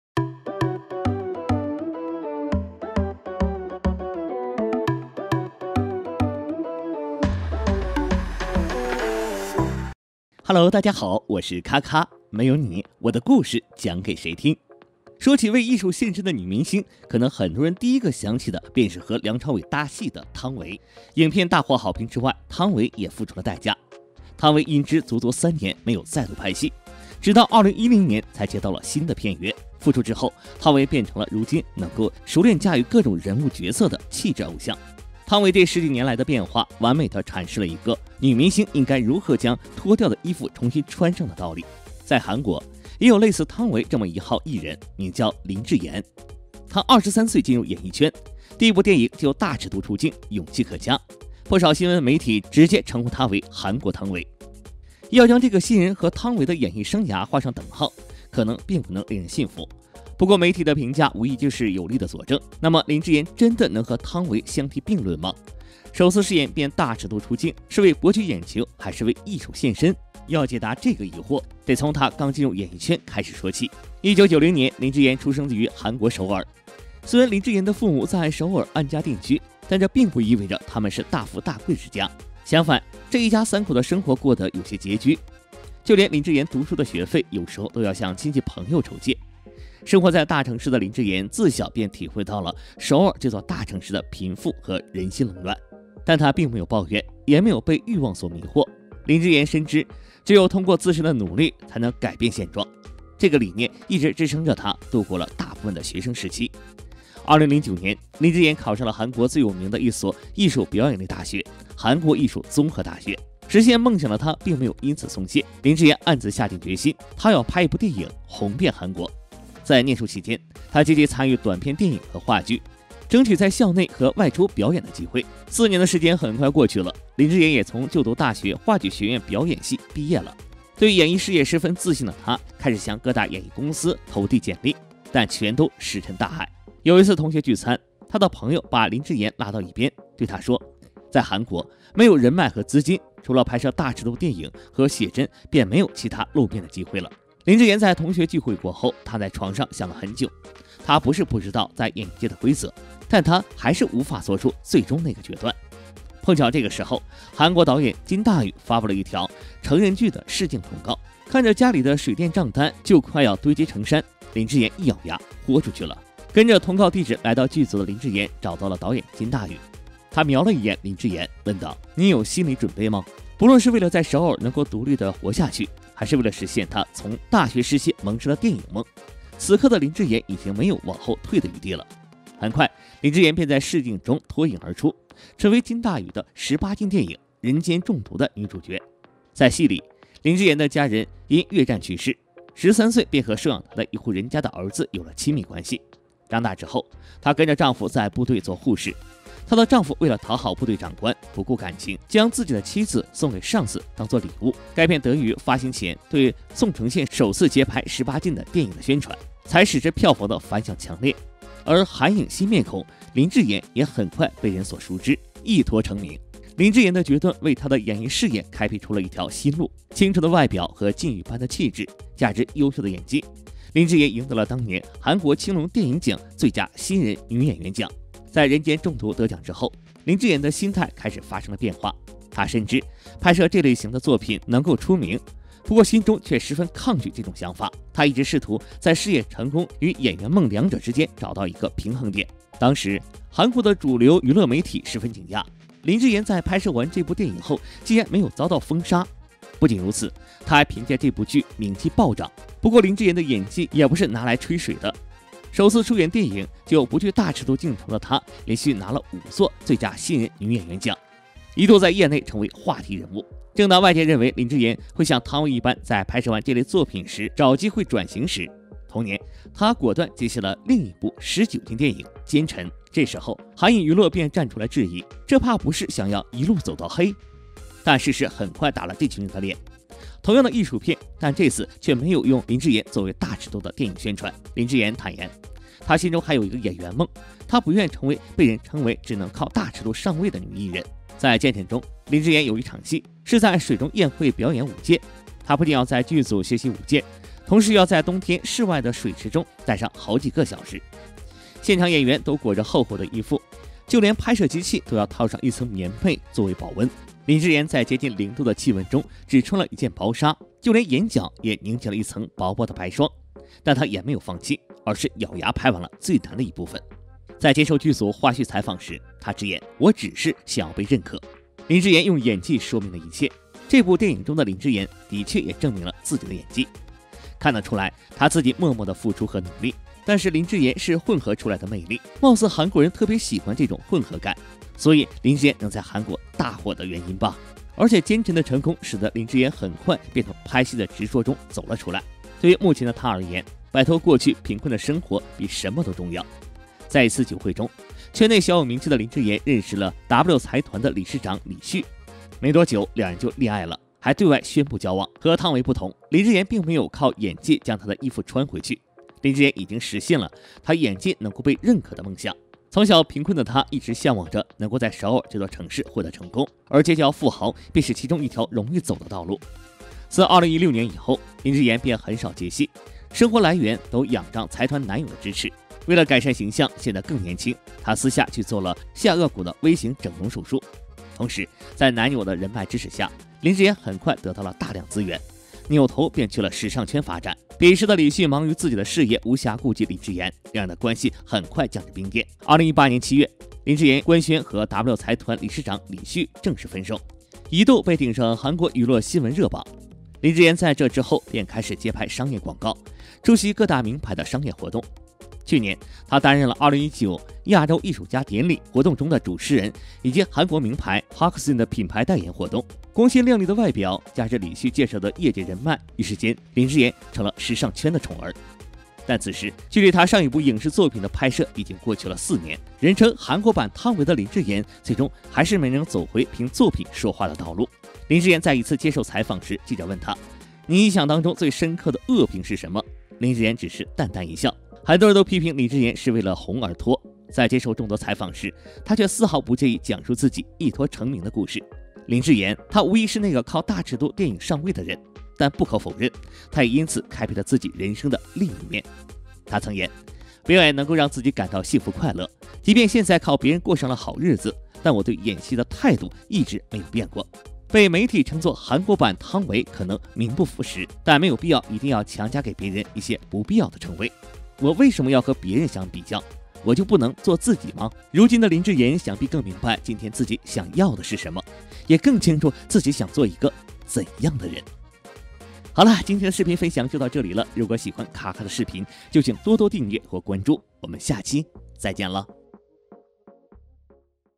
小時候讀書學費全靠借？長大後一脫成名家財萬貫，林智妍直言自己永不後悔！林智妍
坦言想和「朴寶英」談戀愛，被光熙吐槽，戲裏戲外都是富二代得朴炯植曾經曆過什麼？
曾全員遭蹊蹺車禍，還把泫雅踢出局，Wonder Girls的毀滅，不只是因為朴振榮的失敗。